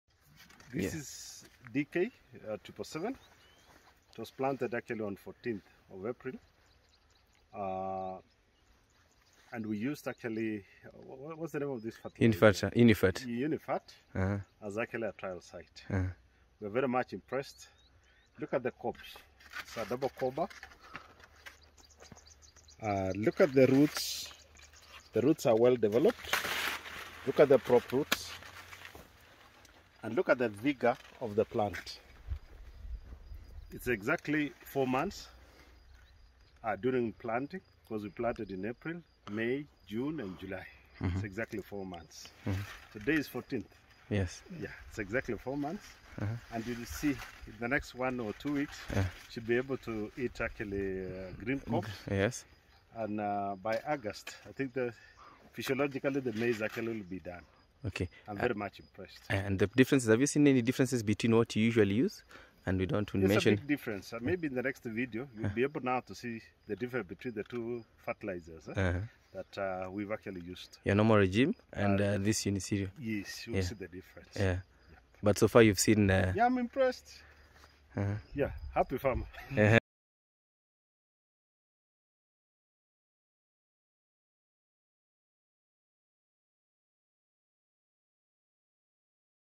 Unicereal Fertilizers (Planting and Top dressing) - Endebess, Trans Nzoia
An honest, unadulterated review from our trial farmer, Mr. Korros from Endebess, Trans Nzoia County who used Unicereal Compound Fertilizers (Planting and Top dressing) in his maize farm. He talks about the performance of the fertilizers as well as overall crop attributes.